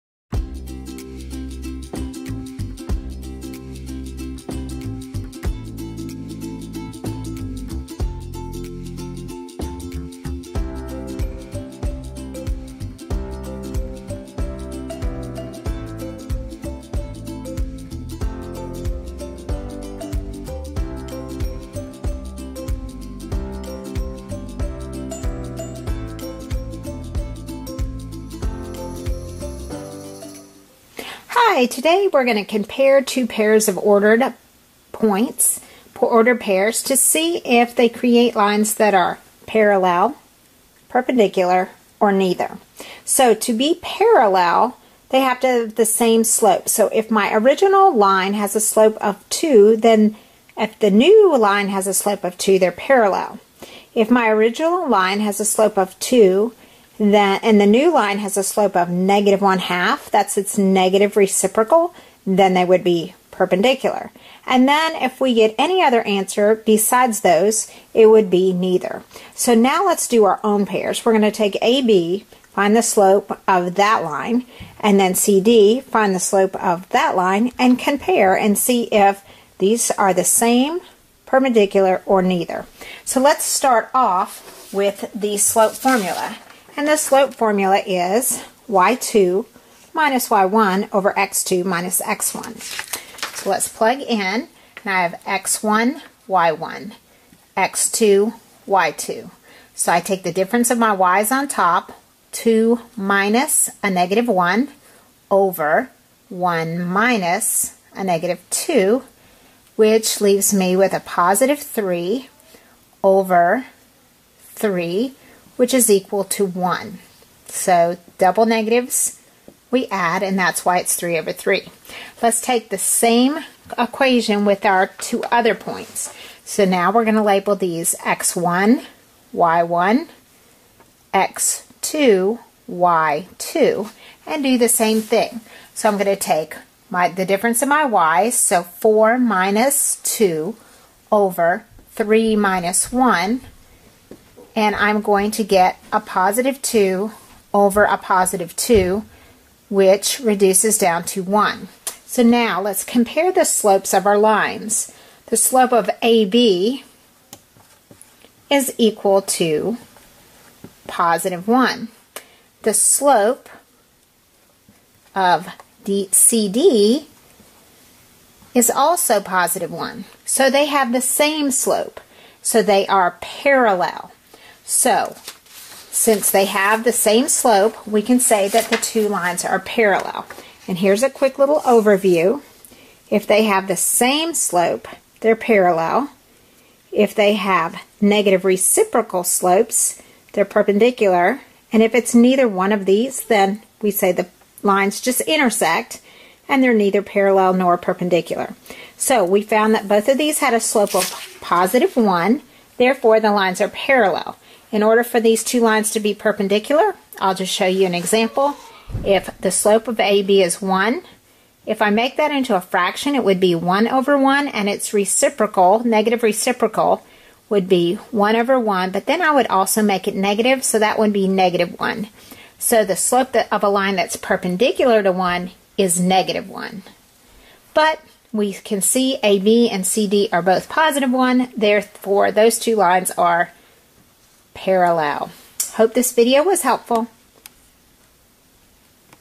Parallel, Perpendicular, or Neither from Ordered Pairs
Given the ordered pairs of numbers (-2,-1) and ( 1,2) how do you tell if they are parallel, perpendicular, or neither?
You can use the slope formula to find the slope and if the two slopes are equal then they are parallel, if they are reverse reciprocal then they are perpendicular and something else they are neither.